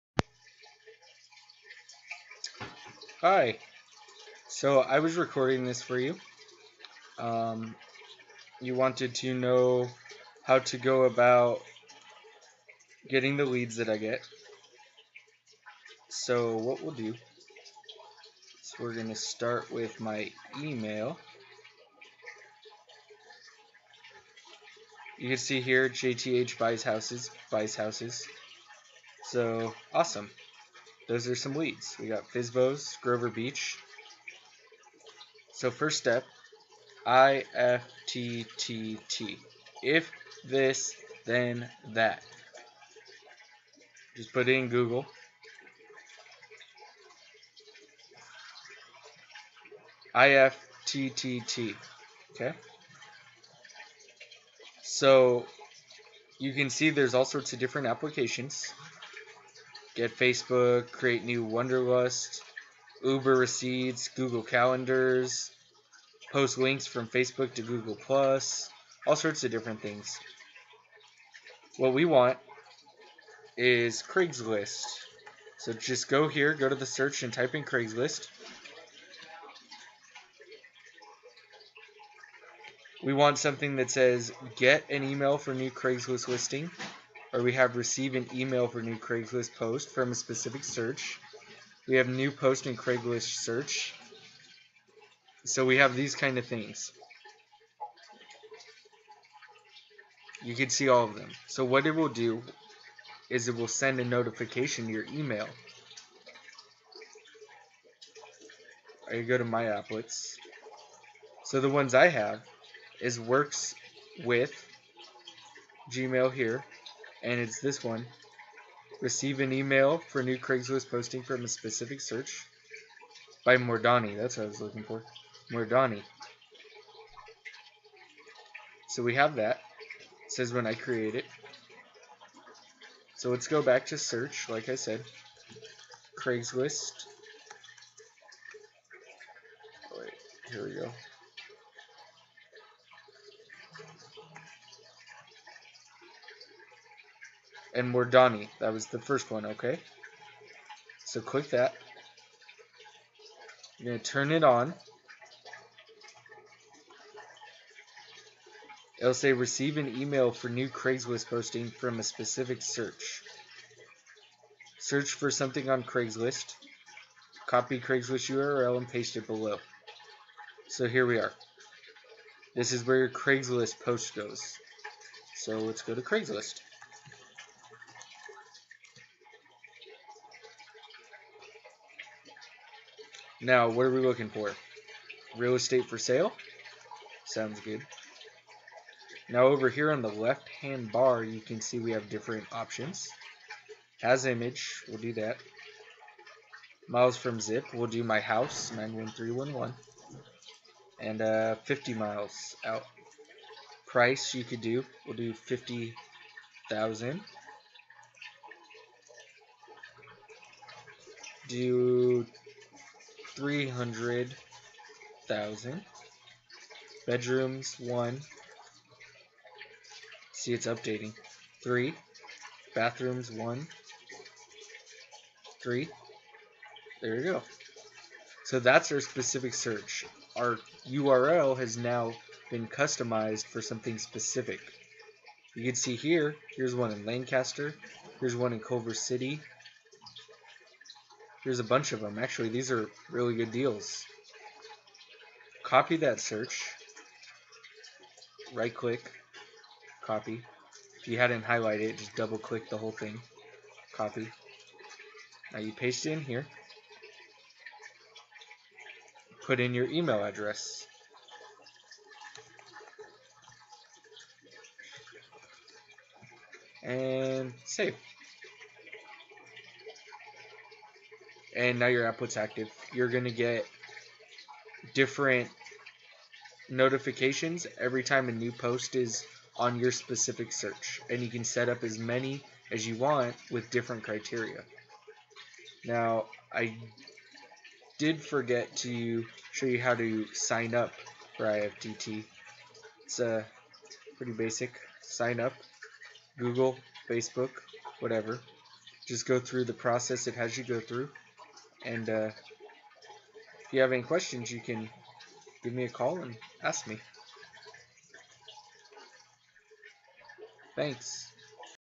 IFTTT Walkthrough for Craigslist Leads
Just a quick walk through of how to setup an applet on IFTTT. Generate leads from craigslist and have them sent to your email! This works for any trade, job, sales program, etc. For more tutorials or videos etc or watch some more videos.